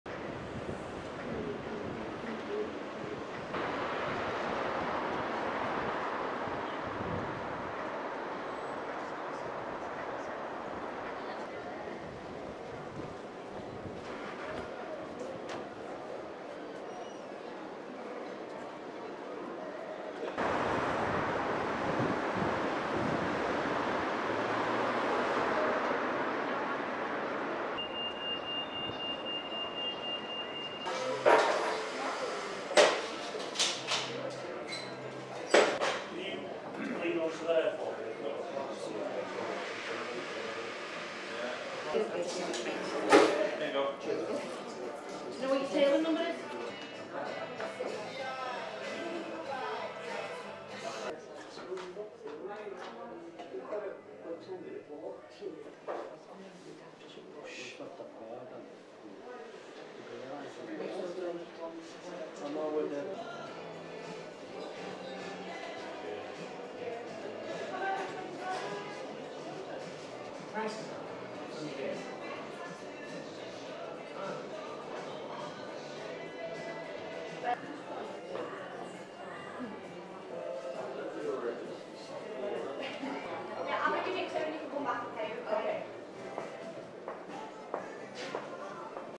شاهد: حانة "البيتلز" في ليفربول تصنف معلماً تاريخياً من الدرجة الأولى
صنفت الأربعاء حانة "فيلارمونيك داينينغ رومز" في ليفربول، حيث كان يطيب لأعضاء فرقة "بيتلز" الأسطورية اللقاء، معلماً تاريخياً. لتكون بذلك أول حانة إنكليزية من العصر الفيكتوري تحوز على هذا التصنيف من الفئة الأولى.…